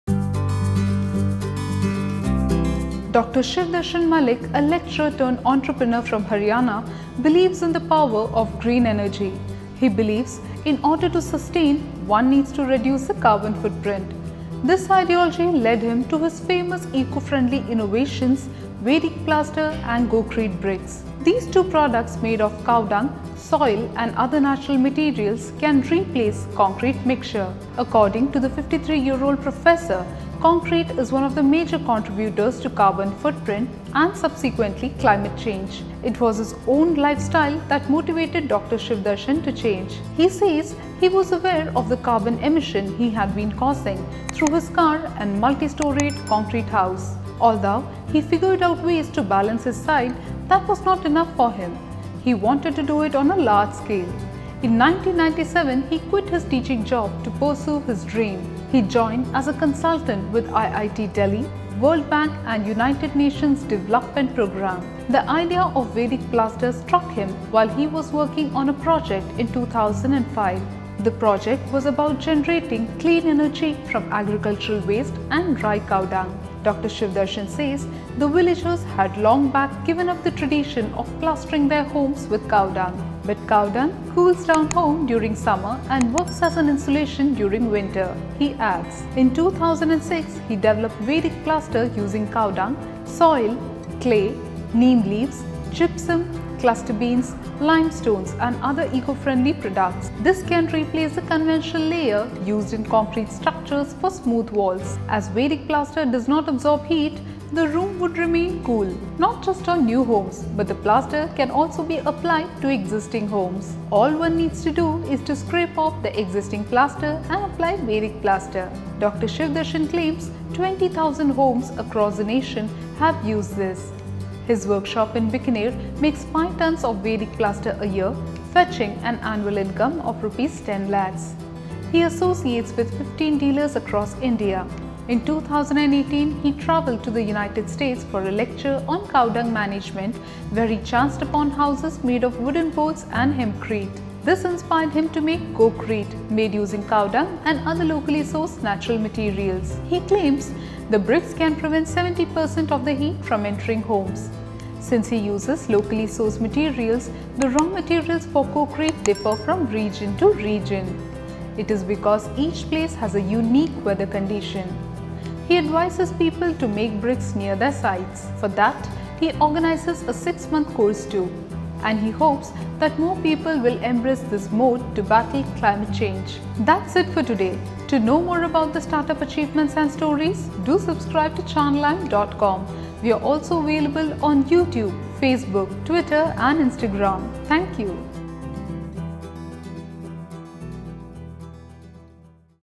'Vedic Plaster' & 'Gocrete Bricks' by Dr Shivdarshan Malik can cool down your home
Dr Shivdarshan Malik, a lecturer-turned-entrepreneur from Haryana, believes in the power of green energy. He believes, in order to sustain, one needs to reduce the carbon footprint. This ideology led him to his famous eco-friendly innovations – ‘Vedic Plaster’ and ‘Gocrete Bricks’. These two products, made of cow dung, soil and other natural materials, can replace concrete mixture. According to the 53-year-old professor, concrete is one of the major contributors to carbon footprint and subsequently climate change.

It was his own lifestyle that motivated Dr Shivdarshan to change. He says he was aware of the carbon emission he had been causing through his car and multi-storied concrete house. Although he figured out ways to balance his side, that was not enough for him; he wanted to do it on a large scale. In 1997, he quit his teaching job to pursue his dream. He joined as a consultant with IIT-Delhi, World Bank, and United Nations Development Programme.

The idea of ‘Vedic Plaster’ struck him while he was working on a project in 2005. The project was about generating clean energy from agricultural waste and dry cow dung. Dr Shivdarshan says, the villagers had long back given up the tradition of plastering their homes with cow dung. “But, cow dung cools down home during summer and works as insulation during winter,” he adds.

In 2006, he developed ‘Vedic Plaster’ using cow dung, soil, clay, neem leaves, gypsum, cluster beans, limestones and other eco-friendly products. This can replace the conventional layer used in concrete structures for smooth walls. As Vedic Plaster does not absorb heat, the room would remain cool. Not just on new homes, but the plaster can also be applied to existing homes. All one needs to do is scrape off the existing plaster and apply Vedic plaster. Dr Shivdarshan claims 20,000 homes across the nation have used this. His workshop in Bikaner makes 5 tons of Vedic Plaster a year, fetching an annual income of Rs 10 lakhs. He associates with 15 dealers across India.

In 2018, he travelled to the United States for a lecture on cow dung management where he chanced upon houses made of wooden boards and hempcrete. This inspired him to make Gocrete made using cow dung and other locally-sourced natural materials. He claims the bricks can prevent 70 per cent of the heat from entering homes. Since he uses locally-sourced materials, the raw materials for Gocrete differ from region to region. “It is because each place has a unique weather condition.” He advises people to make bricks near their sites. For that, he organises a six-month course, too. And, he hopes that more people will embrace this mode to battle climate change.